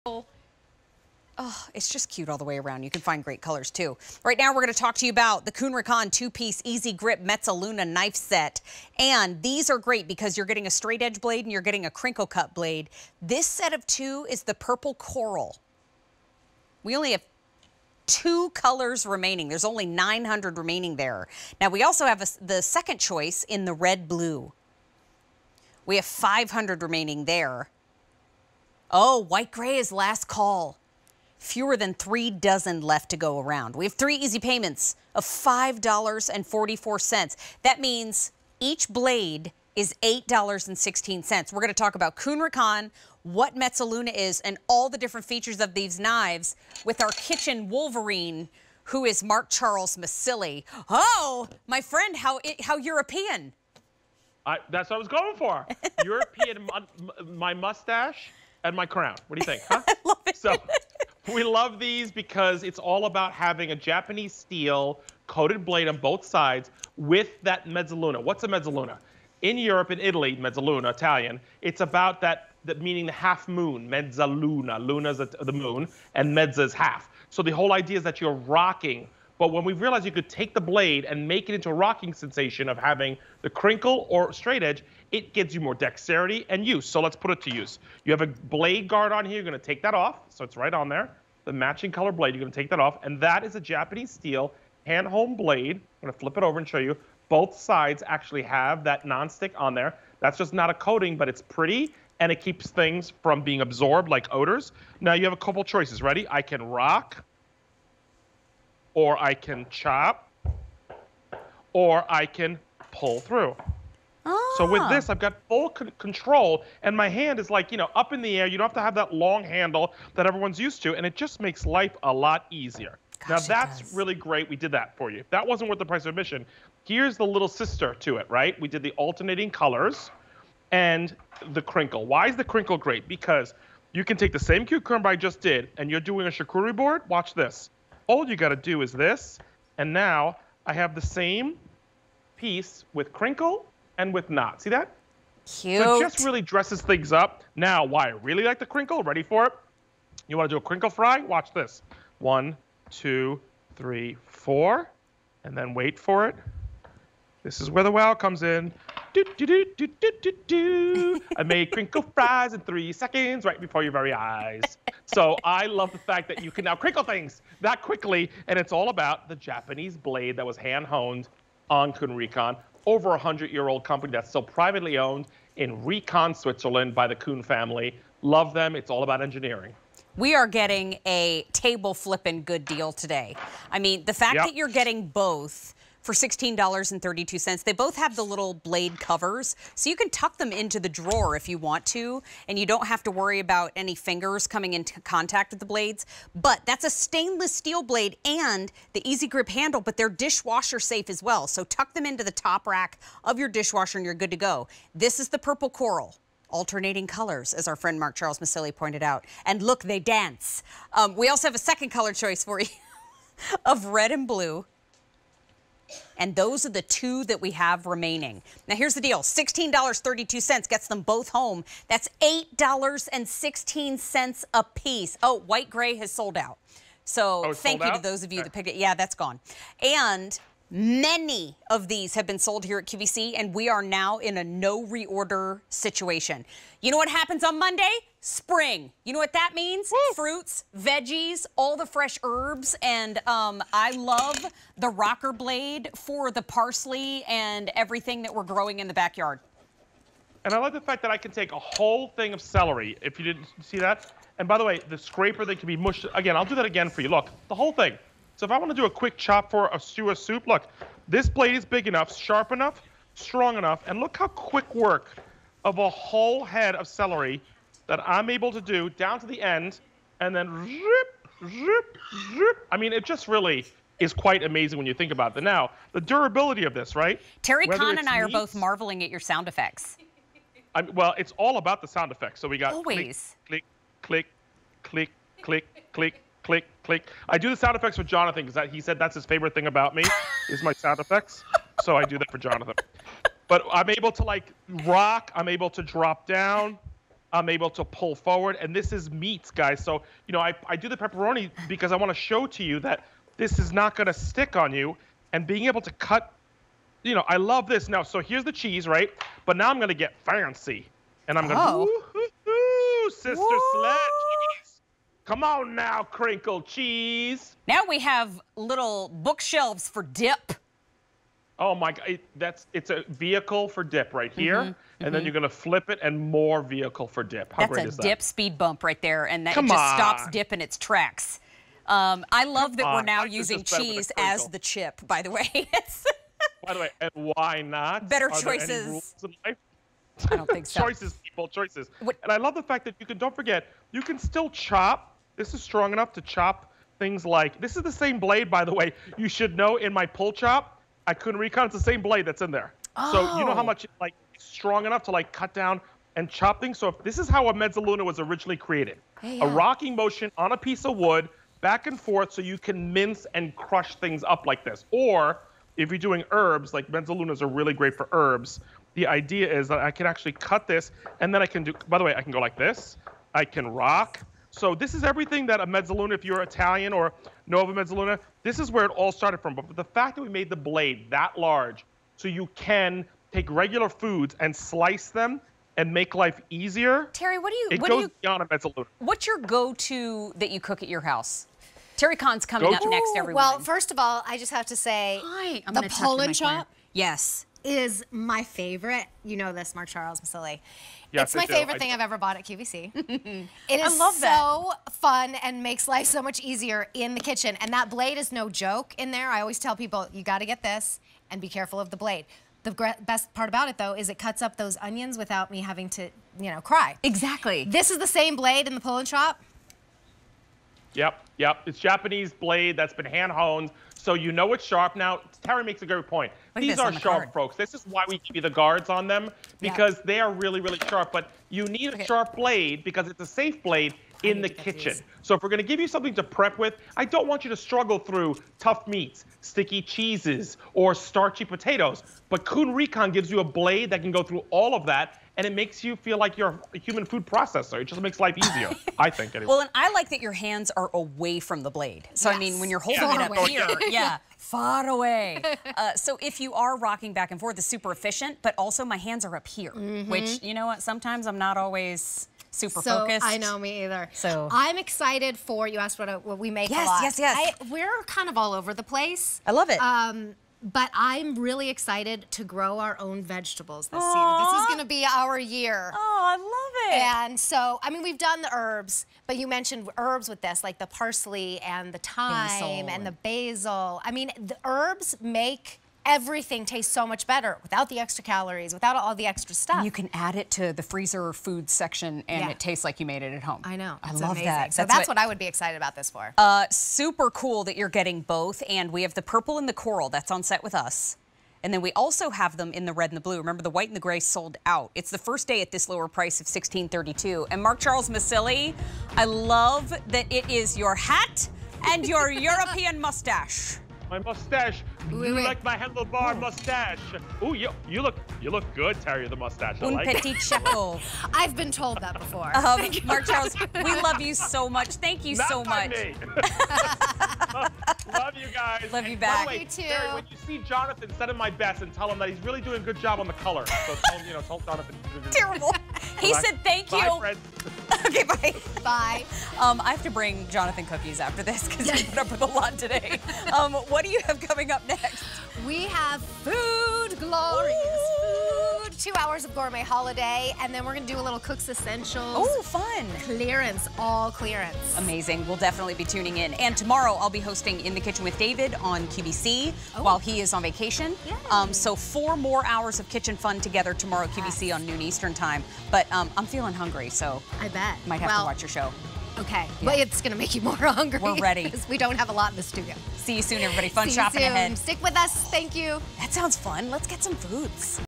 Kuhn Rikon 2-Piece Easy Grip Mezzaluna Knife Set on QVC
Kuhn Rikon 2-Piece Easy Grip Mezzaluna Knife Set

Sharpen your slicing, dicing, and chopping skills with these easy-grip knives. Use the crinkle knife to cut vegetables with a wavy finish such as French fries, carrots, raviolis, and more. The mezzaluna knife makes quick work of chopping herbs and smaller vegetables. From Kuhn Rikon.